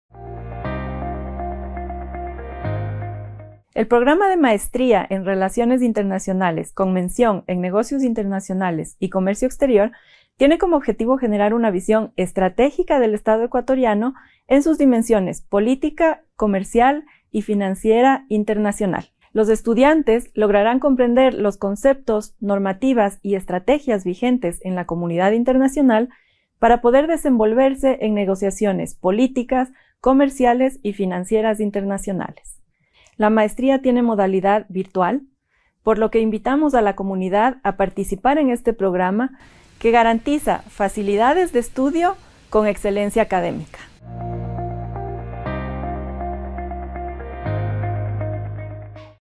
Maestría en Relaciones Internacionales - Oferta Académica Octubre 2022
La Escuela de Relaciones Internacionales, del Instituto de Altos Estudios Nacionales - IAEN, invita a postularse a la Maestría en Relaciones Internacionales Mención en Negocios Internacionales y Comercio Exterior.